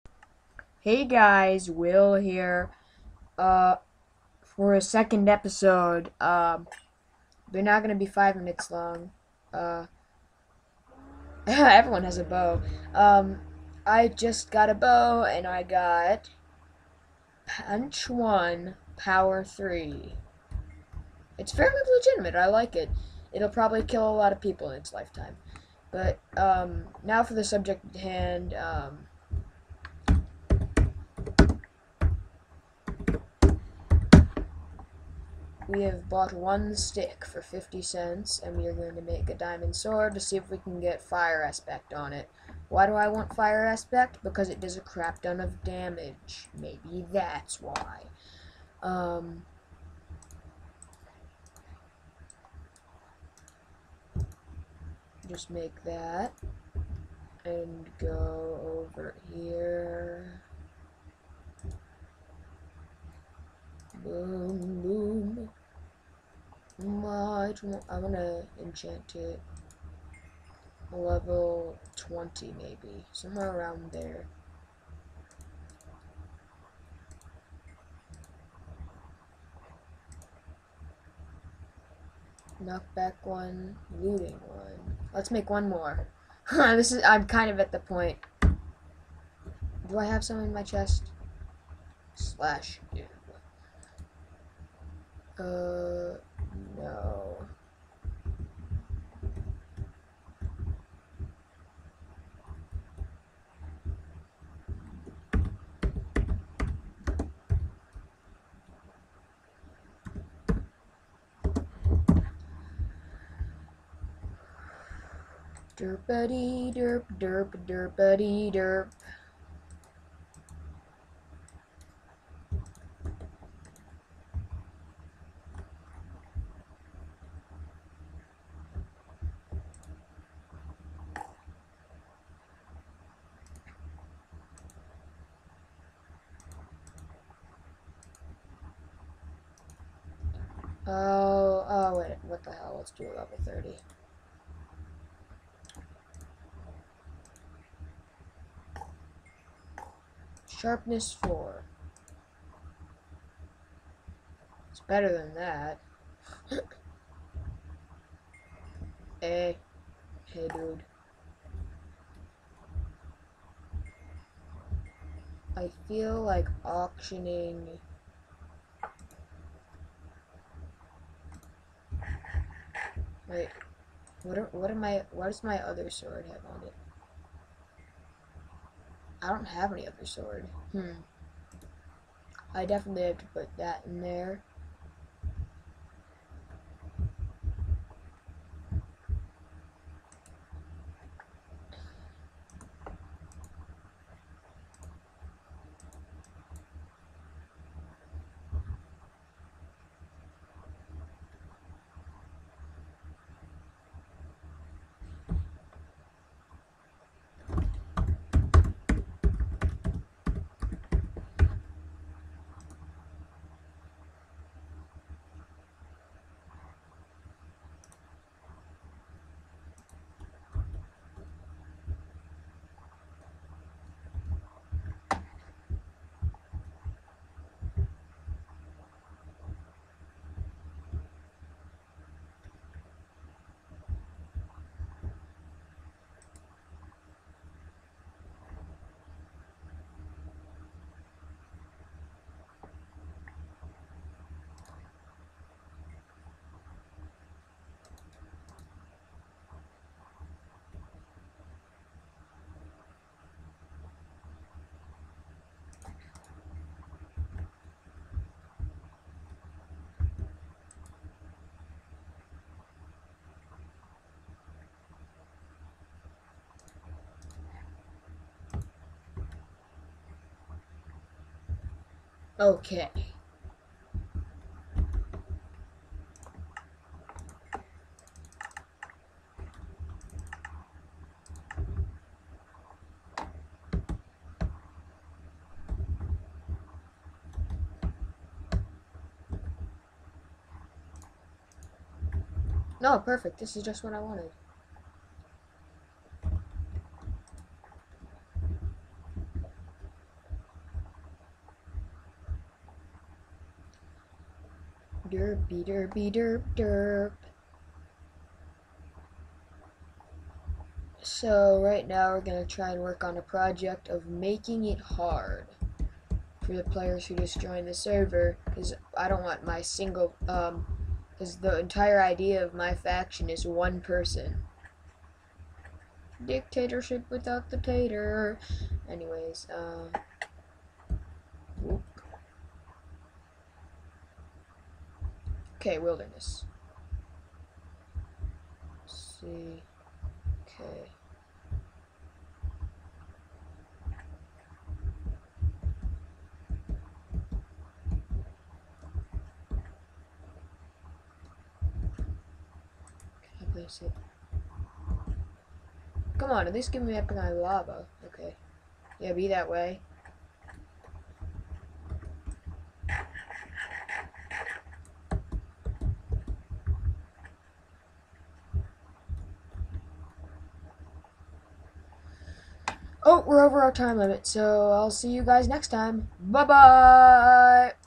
WillPlaysFactions-Episode 2..... Setting The Traps(Live Commentary, And No More Crappy Graphics)
I've decided not to do a Let-Play. But I am doing an episode a day or every other day, it depends on my schedule.